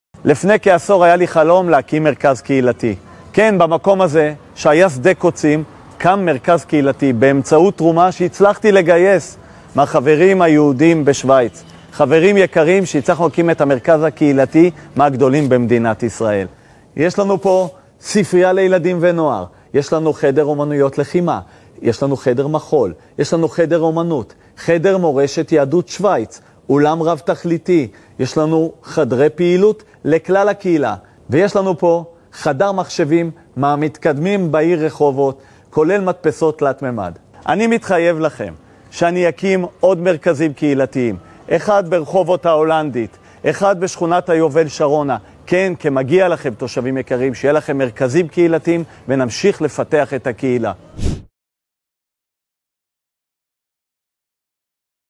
חדשות 08 | ההתחייבות של זהר בלום לתושבי רחובות
"הגיע הזמן לתת לתוצאות שלי בשטח לדבר בעד עצמן" - אומר ממלא מקום ראש עיריית רחובות והמועמד לראשות העיר זהר בלום ומוסיף: "העשייה מנצחת".

בסרטון שהפיץ לעשרות אלפי עוקביו ותומכיו ציין: "יזמתי והקמתי מכספי תרומות שגייסתי את שני המרכזים הקהילתיים הגדולים בעיר, מתנ״ס חוויות שוויץ צפון ומתנ״ס רחובות החדשה שמעניקים שירות מגוון וחשוב לילדים, נוער והמשפחות בשכונות.

אני מתחייב כראש עיר להקים מרכזים קהילתיים דומים בשכונת רחובות ההולנדית, בשכונות היובל-שרונה ובכל שכונה שבה יהיה צורך אמיתי של הקהילה. צפו בסרטון ותראו איך הופכים שדה קוצים לפנינה של השכונה".